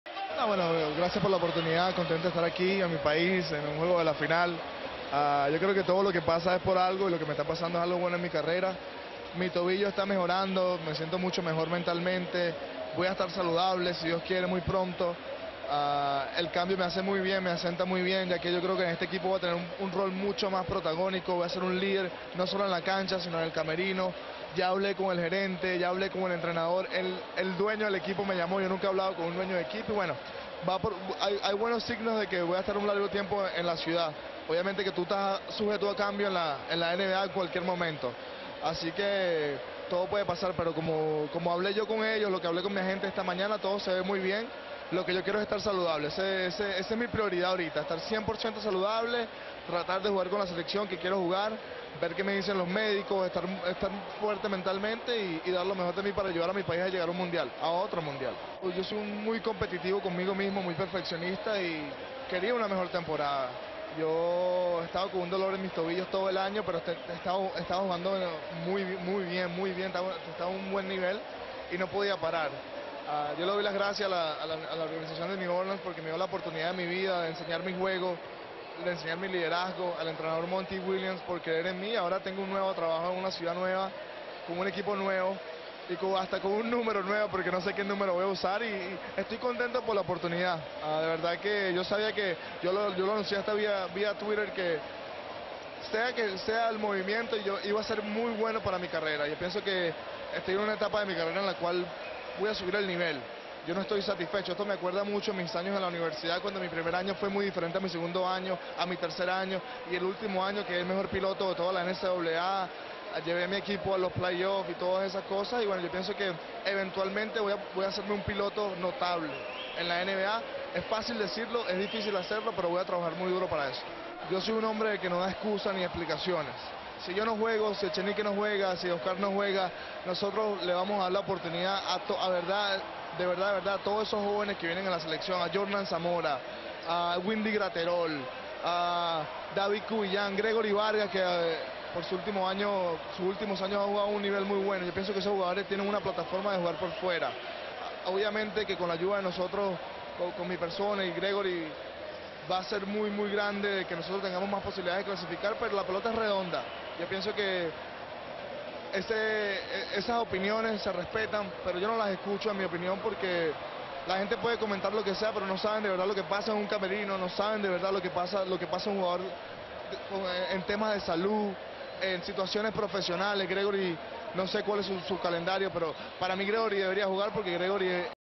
Greivis Vásquez: "Voy a estar saludable muy pronto"
El criollo comentó que cree que va a estar mucho tiempo con su nuevo equipo en la NBA, los Reyes de Sacramento